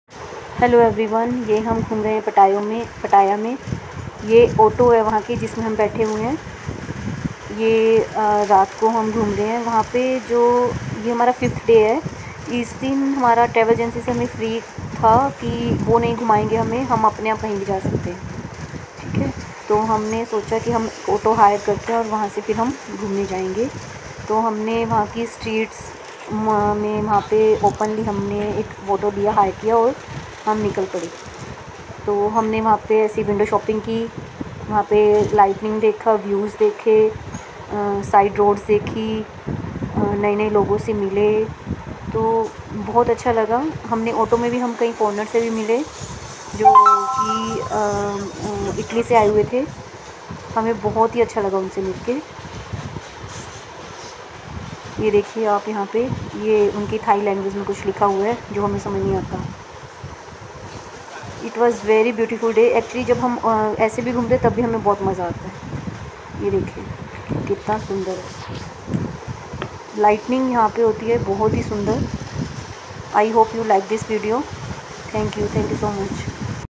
Thailand Trip Day 5 : Pattaya Streets at Night
This is Day 5 of my Thailand Trip. We are exploring Pattaya Streets at night today on Auto. It is very reasonable and we met a lot of foreigners on it as well.

Day 5 - Pattaya Streets at Night